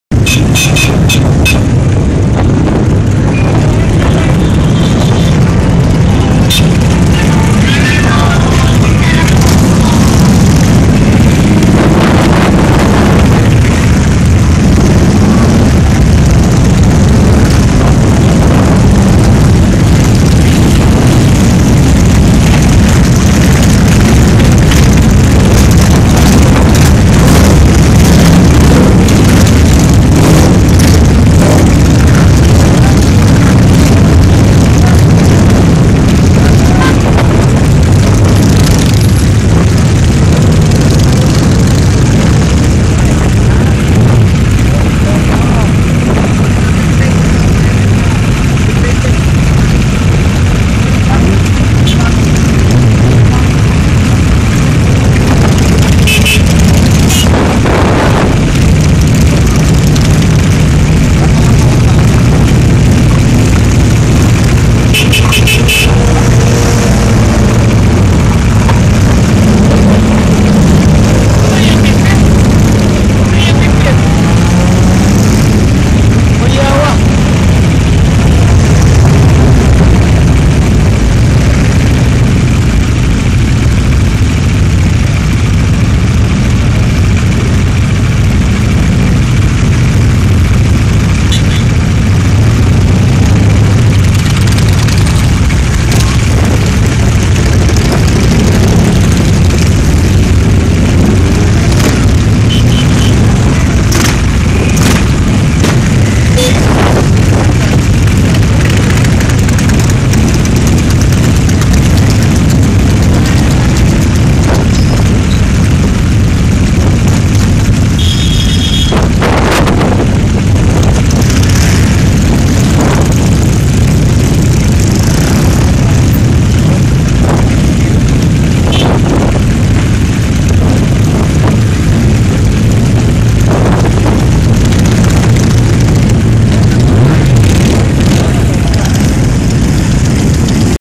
2013 Cebu Sinulog Vibration Run
Sinulog Vibration Run is an big bike annual event in line with Sinulog Celebration. This is alos on the same day with the Santo Nino procession. Most of the major road in Cebu City is closed on this day that is why the bike rally is not on the city but going towards south to the City of Naga.